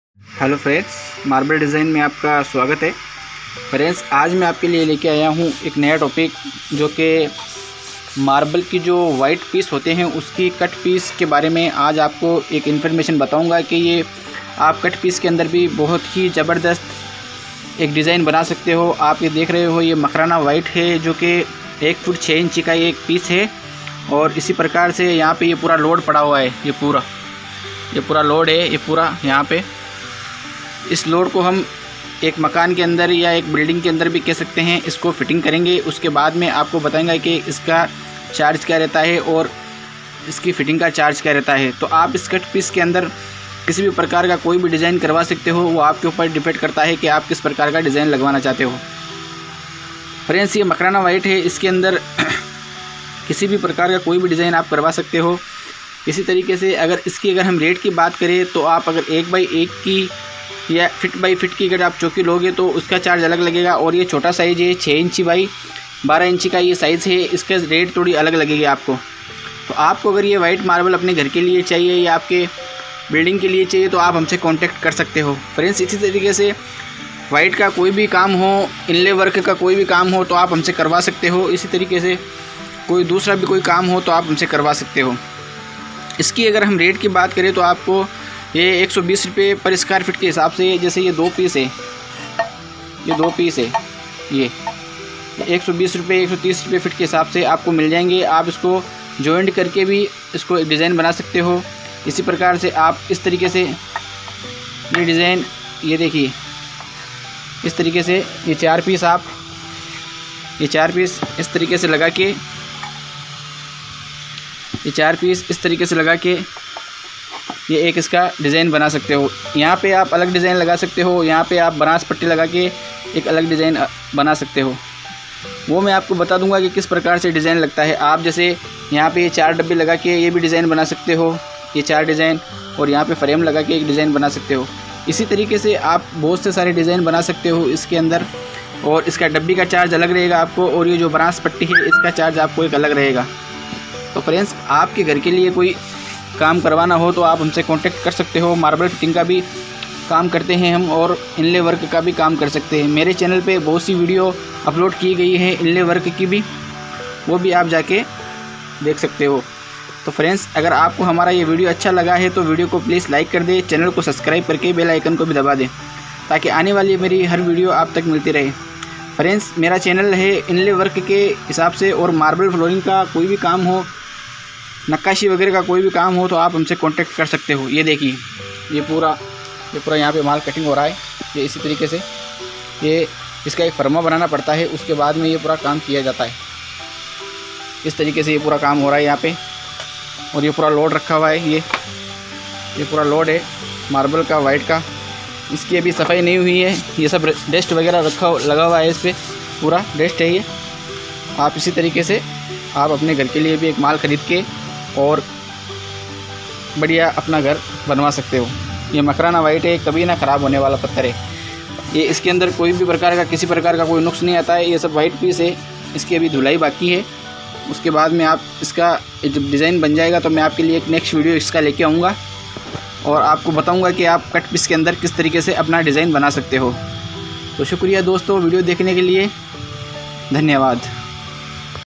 White tile makrana cut piece /marble design
Friends agar aap humse Koi kam karvanachahte to aap ham se sampark kar sakte ho hamne aapko is vivio me bataya he ki aap aapke garme kis tarhase marbe laga sakteho or aapko  marbal ka Kam karvana he to sampark kare or inlay work ka bikam karva saktehe